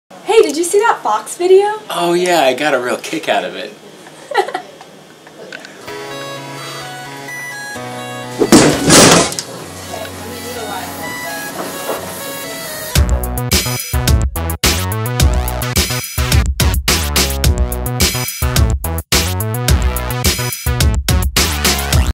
Get a Kick Out of It
When an American tells you that they “got a kick out of” something, they don’t mean that they received an actual kick. Instead, an American is telling you that he or she found something amusing or funny.

* This video is part of an ongoing series, produced by the University Writing Center at Texas A&M. The project is intended to help students understand the meaning of popular American idioms and appreciate the use of figurative language.